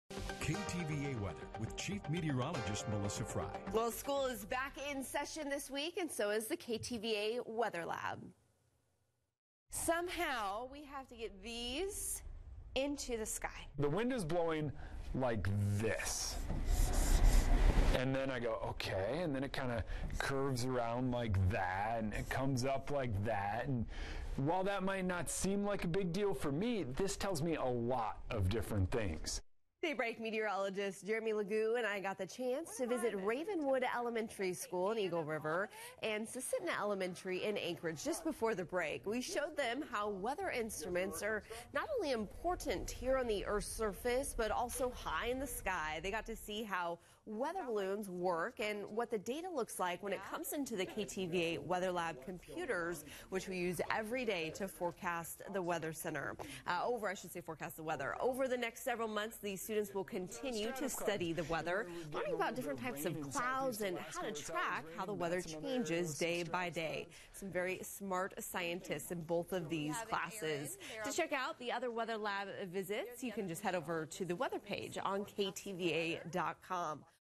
KTVA Weather Lab: Ravenwood and Susitna Elementary Schools
KTVA's Meteorologists Melissa Frey and Jeremy Lagoo visit Ravenwood and Susitna Elementary Schools to teach students about weather instruments and how they help meteorologists forecast the weather.